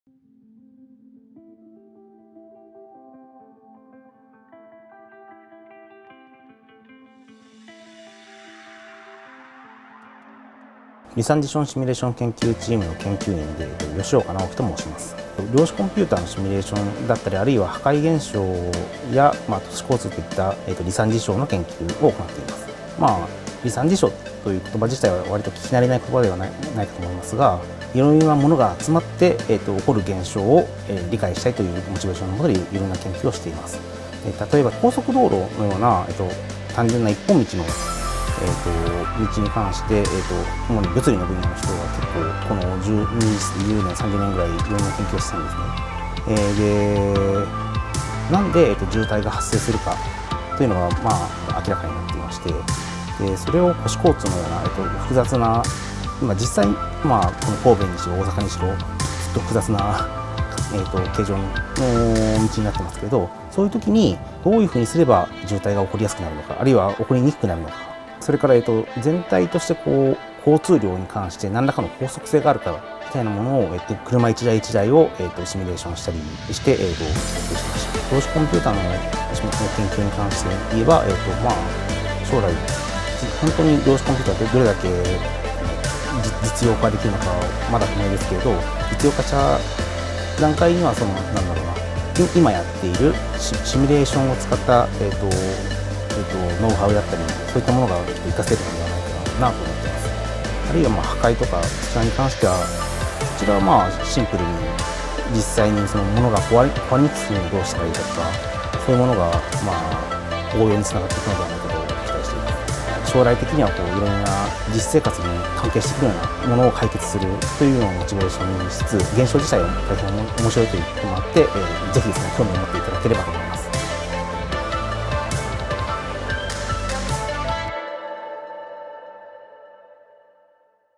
量子コンピュータや離散事象に関するシミュレーションの研究事例
京コンピュータや富岳を用いて行っている量子コンピュータの大規模シミュレーションについて紹介します。また、離散事象シミュレーションの研究事例として、交通流や破壊現象の研究を解説します。
出演: 計算科学研究センター 離散事象シミュレーション研究チーム 研究員 吉岡直樹2021年制作。

関連リンク
（1）理研計算科学研究センター離散事象シミュレーション研究チーム：